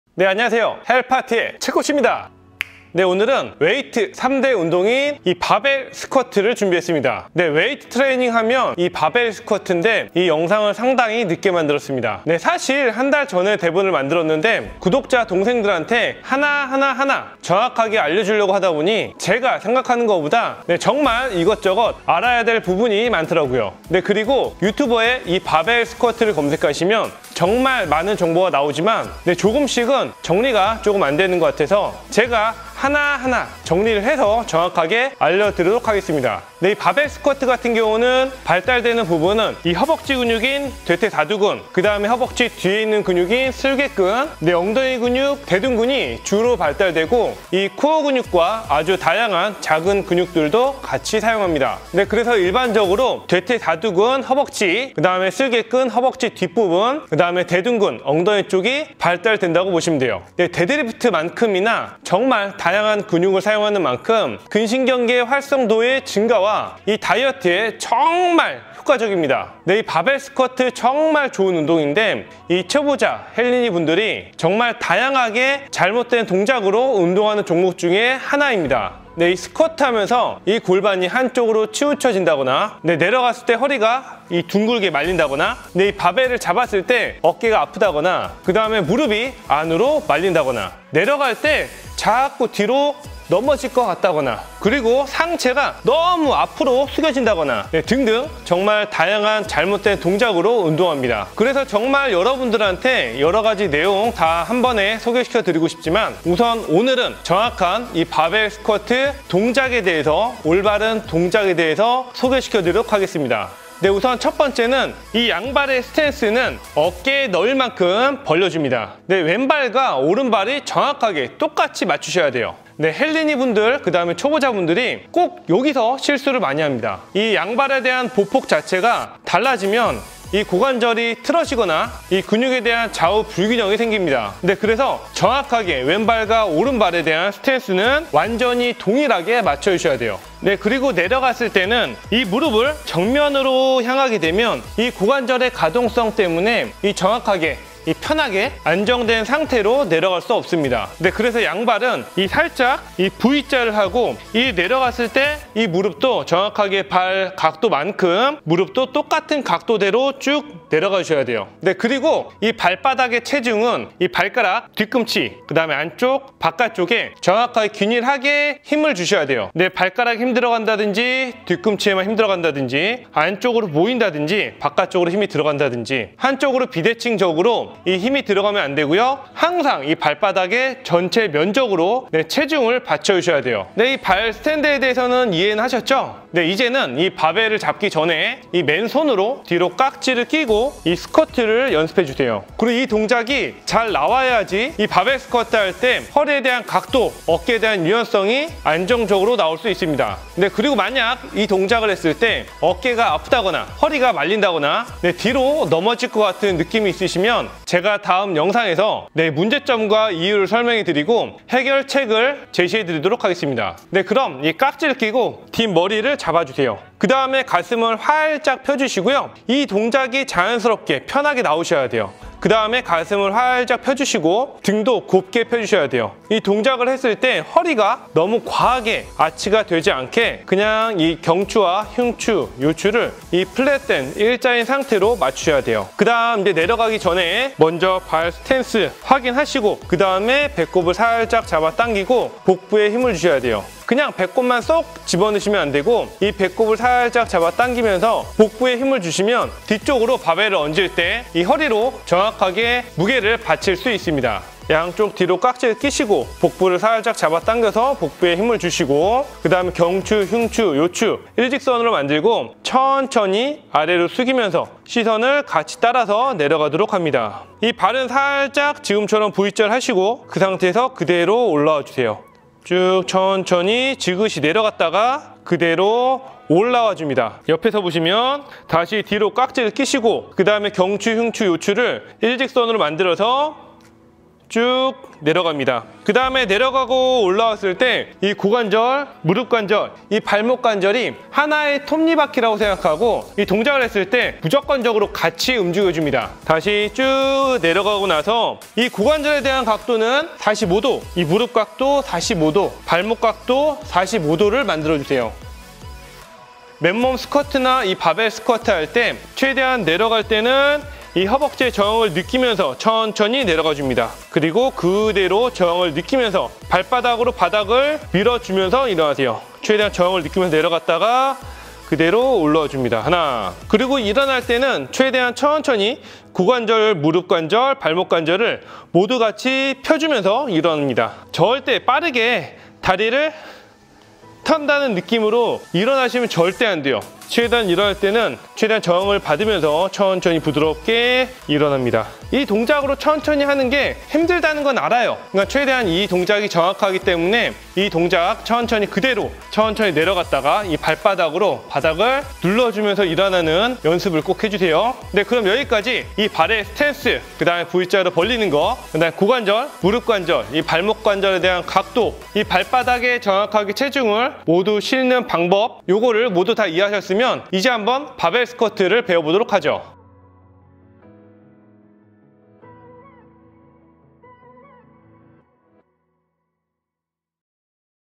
스쿼트의 정석, 초보자를 위한 내용 정리
안녕하세요 헬파티의 최코치 입니다!
오늘은 하체운동인 스쿼트를 준비 했습니다.

정말 초보자들이 꼭 봐야할 스쿼트 내용을 준비 했습니다.
스쿼트 효과와 발달되는 부위에 대해서
설명을 해놨습니다!